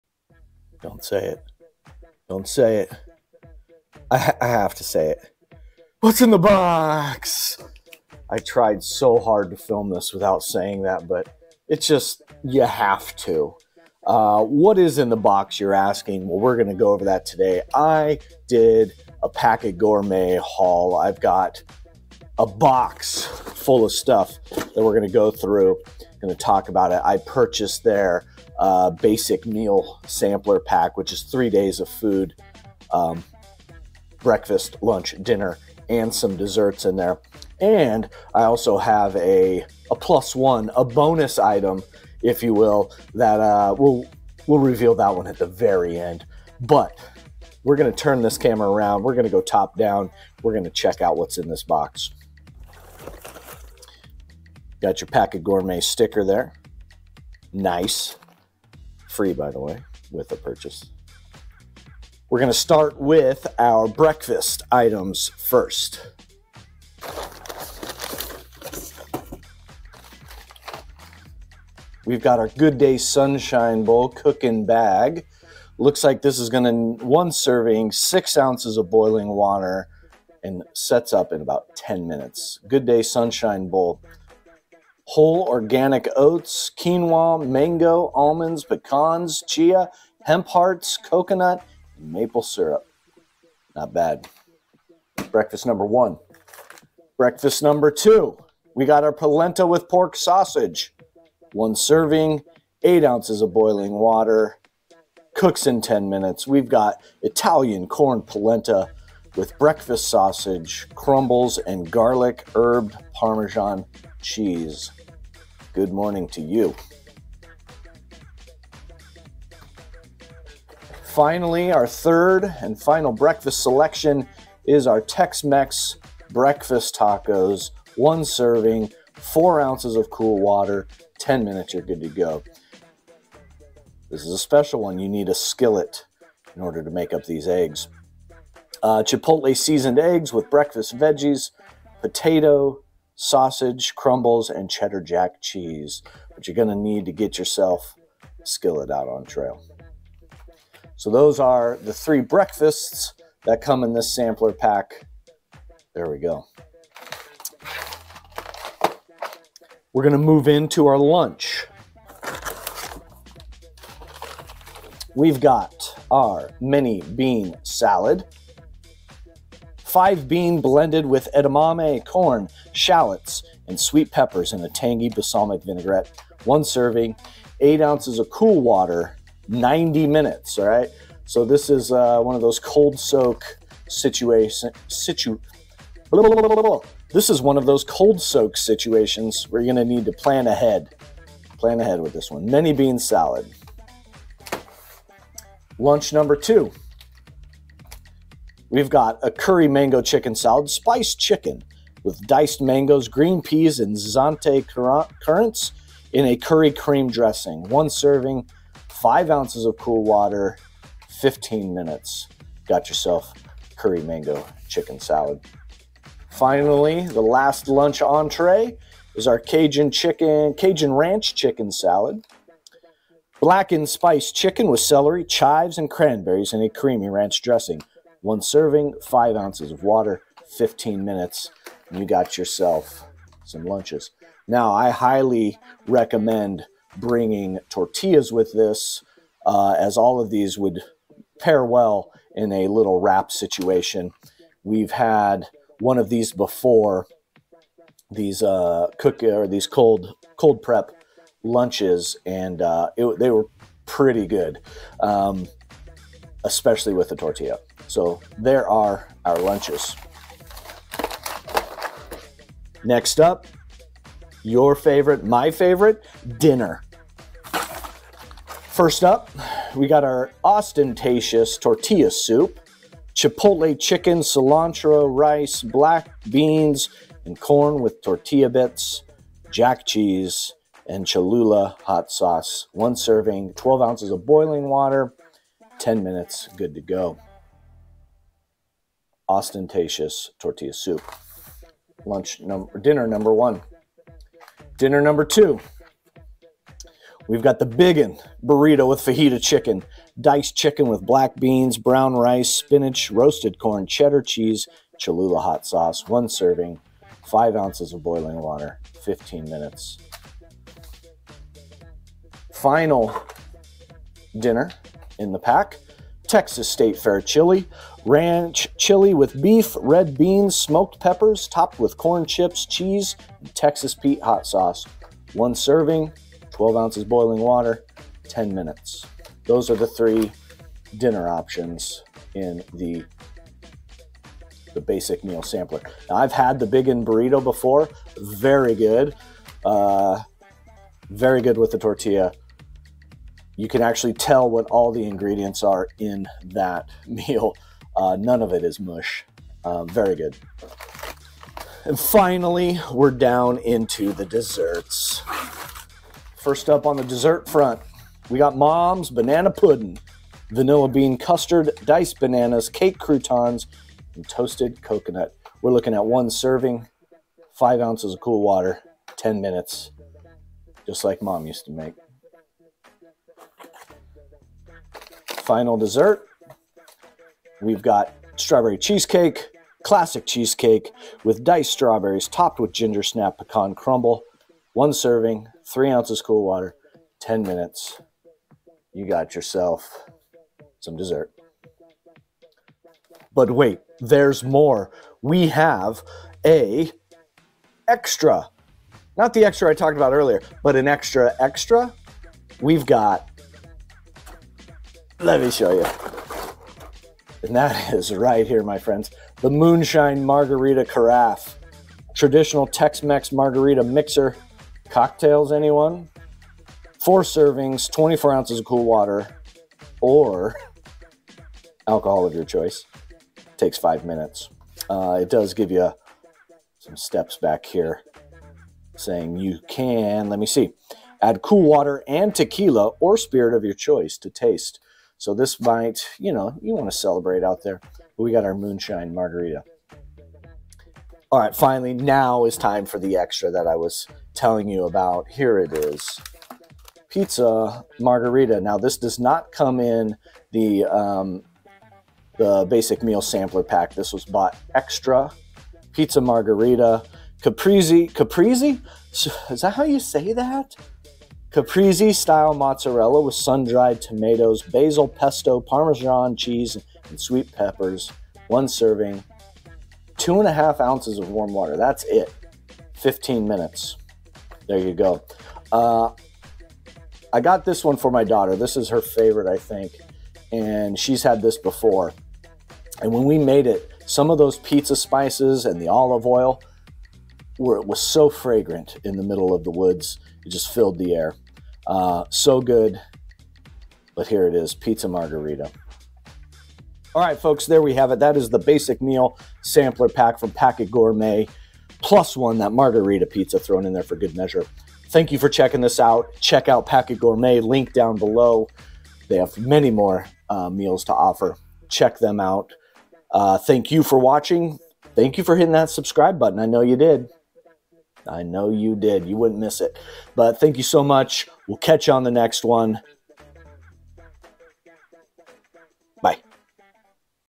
Food Haul | Packit Gourmet -- Basic Meal Sampler | Two-Sox Hikes
Today I breakdown the Basic Meal Sampler pack from Packit Gourmet, plus a bonus meal.